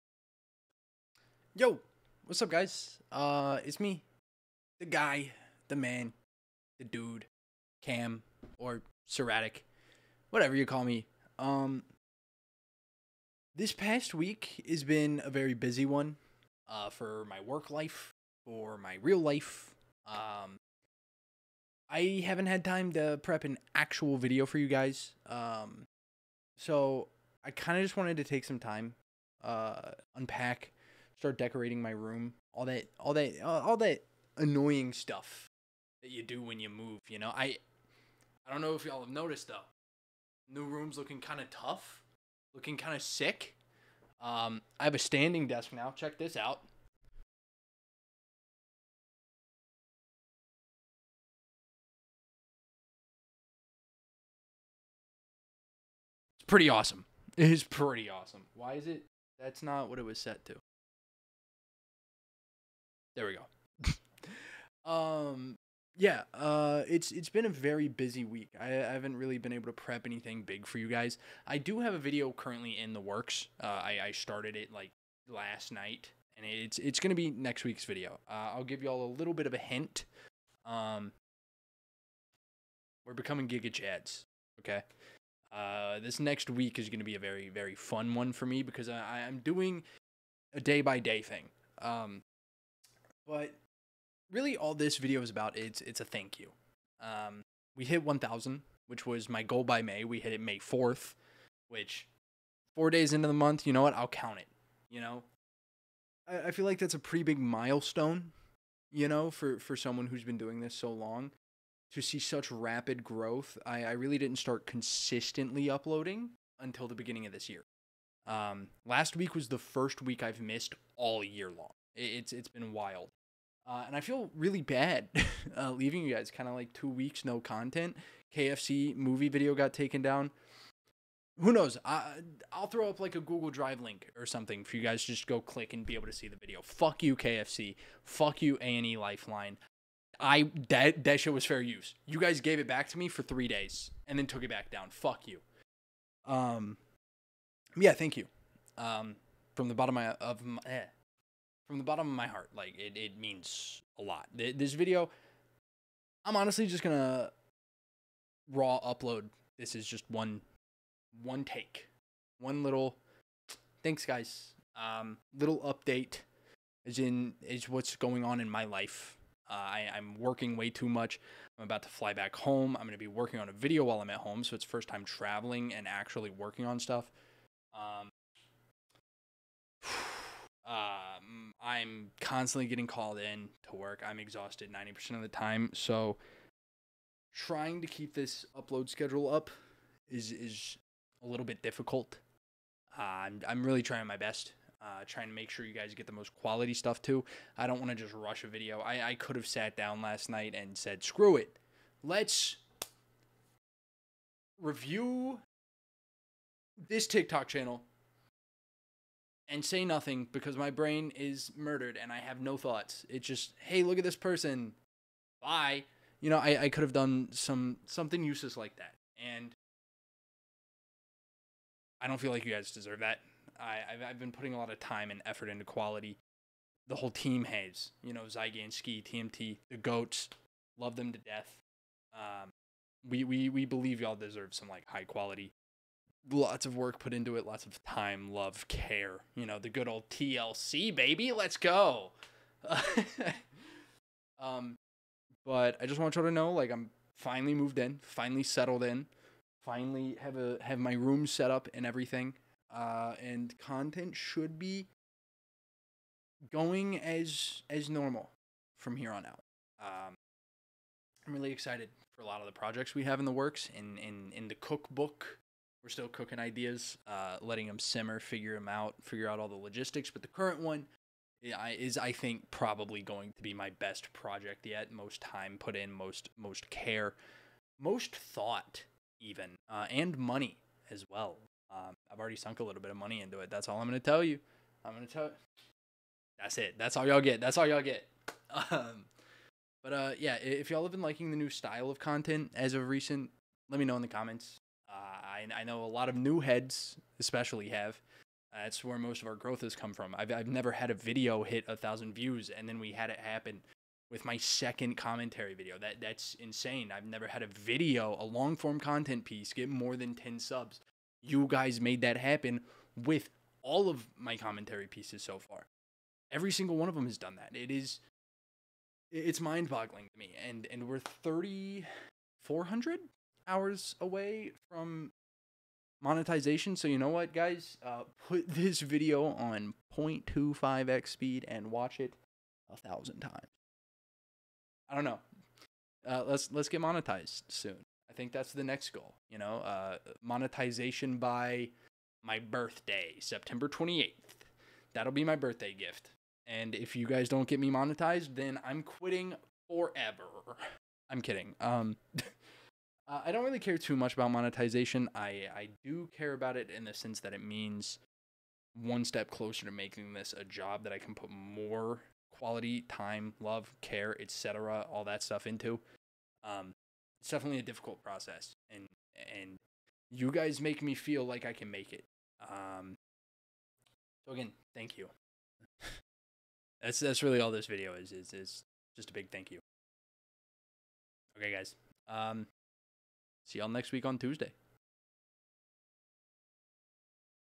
thank you.
thank you guys so much for getting me to 1k, thank you for all of the recent support, the likes subs comments etc etc. it means a lot. and i promise things are gonna keep getting better